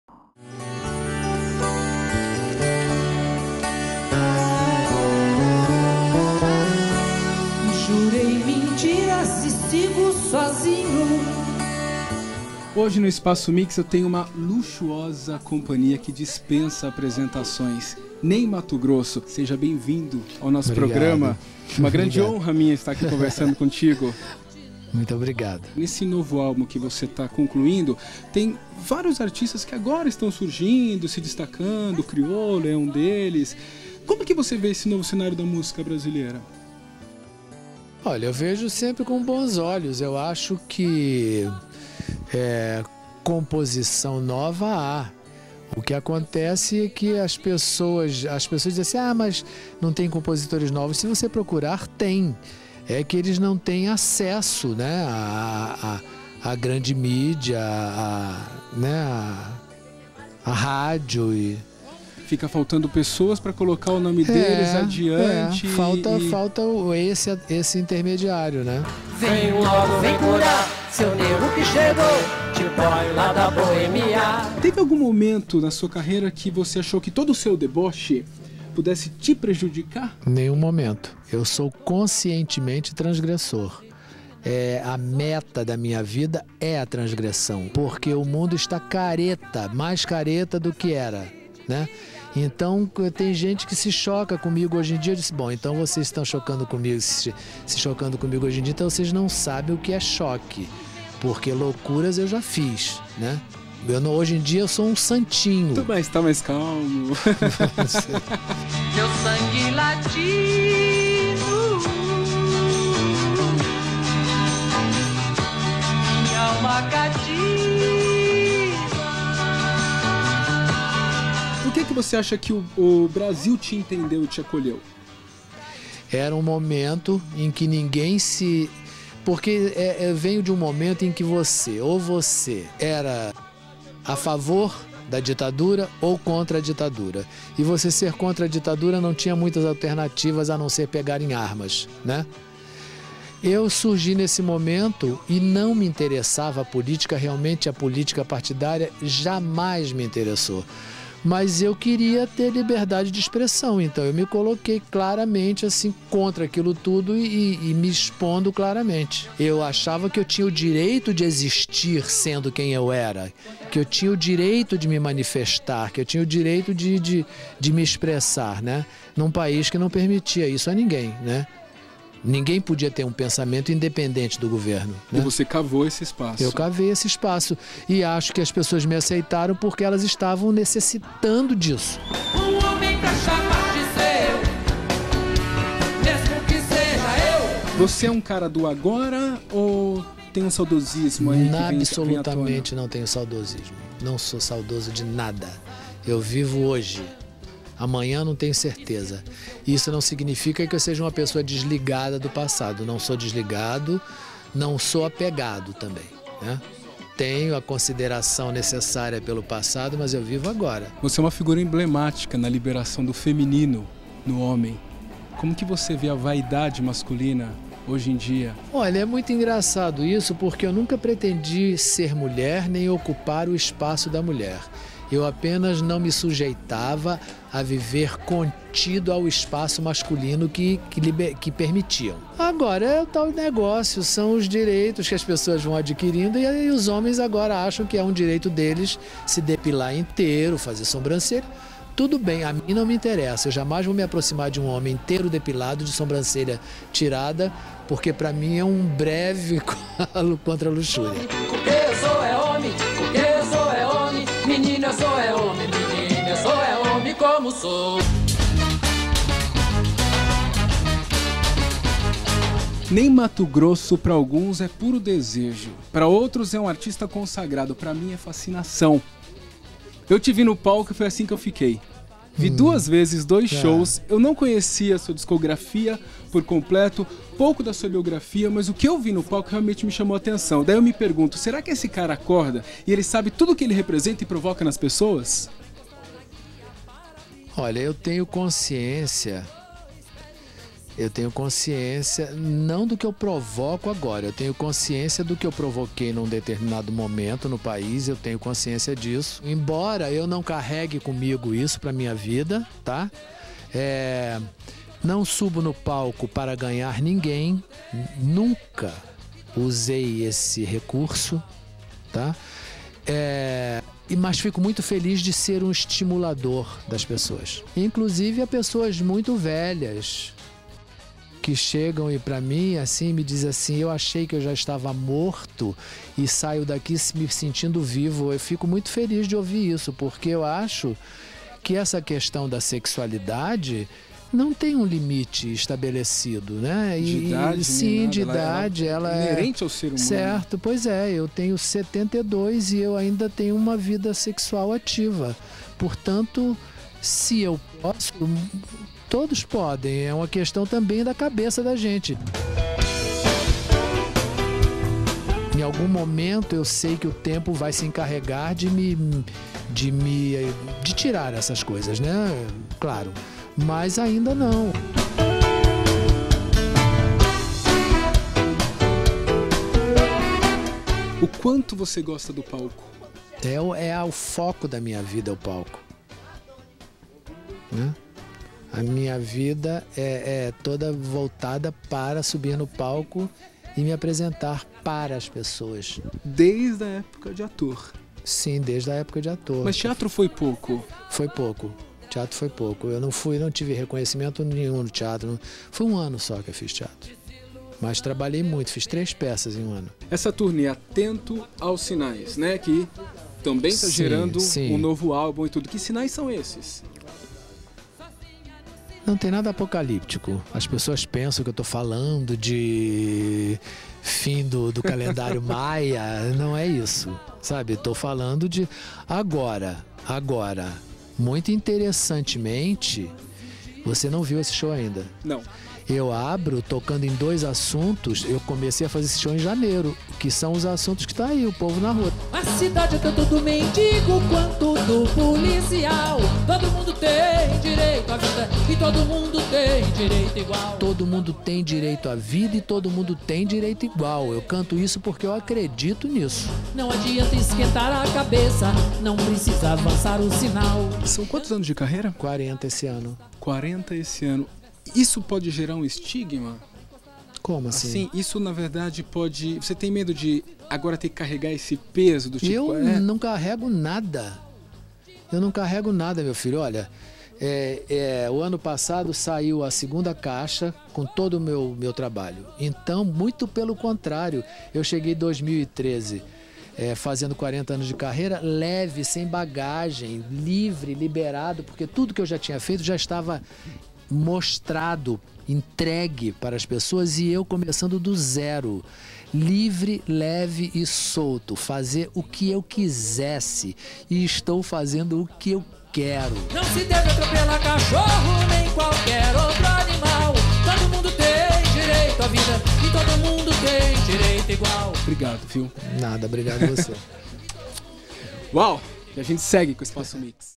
Fabrício Pietro com NEY MATOGROSSO
Entrevista com o cantor na Fazenda Serrinha, quando foi o grande homenageado do Festival de Arte.